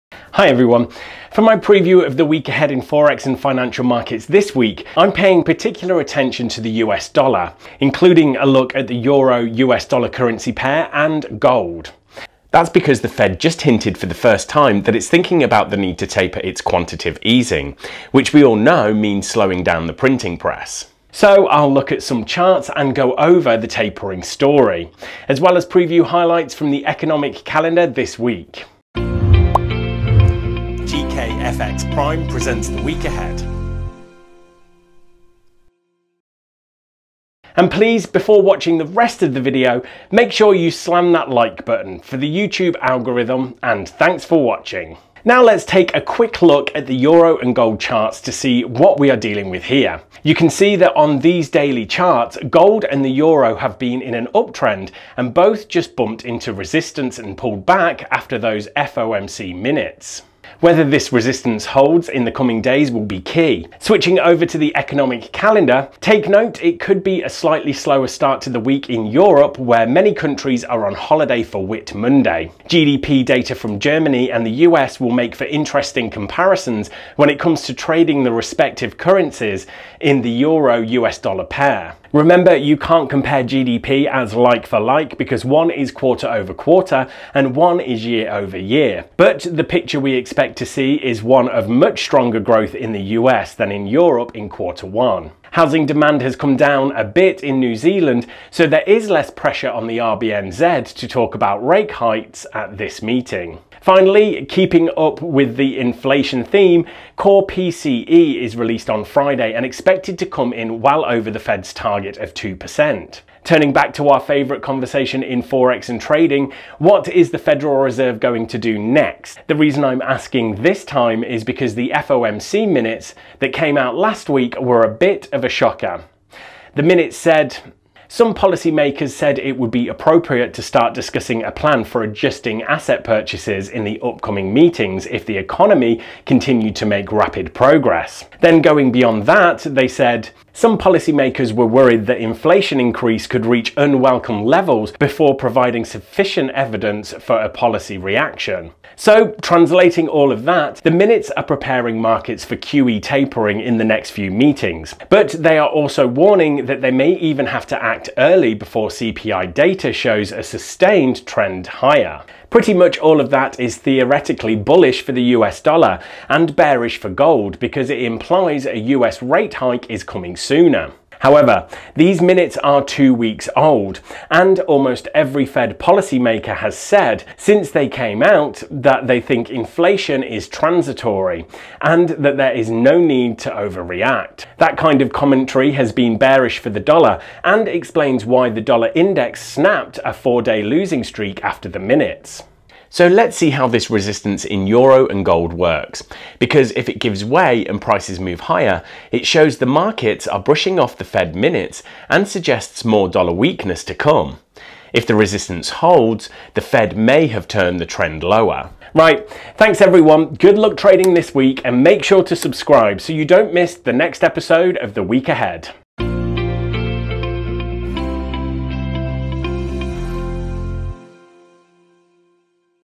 Fed Tapering, Forex & Emas | WEEK AHEAD
Fed telah memulakan perbincangan ringkas tentang tapering jadi saya akan bercakap tentang apakah kesannya terhadap dollar, harga emas dan carta EUR/USD. Saya juga akan menelusuri kalendar ekonomi minggu ini. Terima kasih! Rich

Video ini mempunyai kapsyen tertutup. Sila klik butang CC untuk mengaktifkan sari kata.